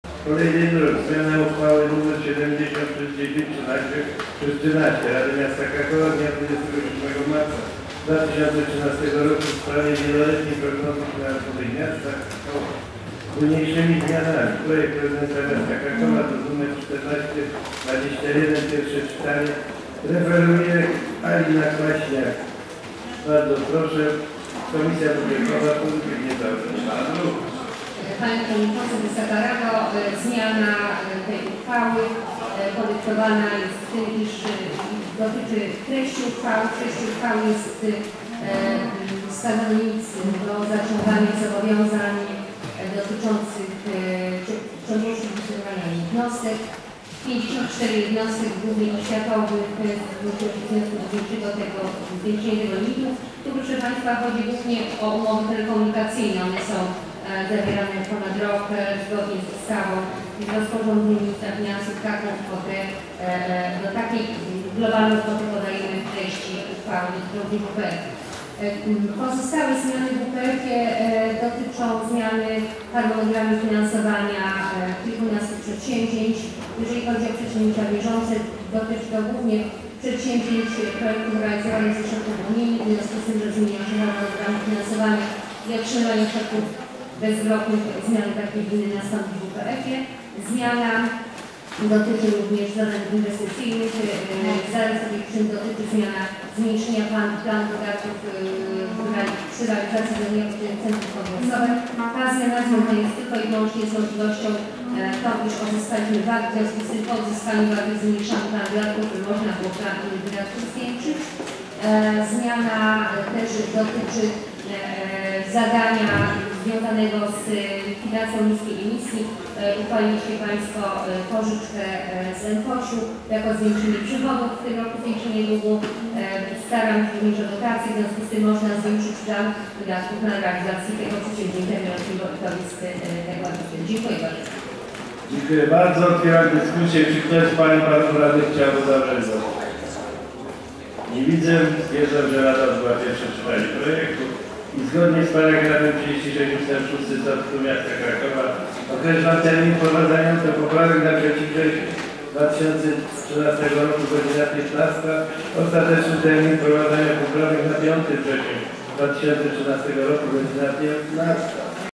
30. Zmiana Wieloletniej Prognozy Finansowej Miasta Krakowa
Zmiana uchwały Nr LXX/1013/13 Rady Miasta Krakowa z dnia 27 marca 2013 r. w sprawie Wieloletniej Prognozy Finansowej Miasta Krakowa (z późn. zm.)
Projektodawca Prezydent Miasta Krakowa, referuje Alina Kwaśniak
Pozytywna opinia prawna Krzysztof Stefański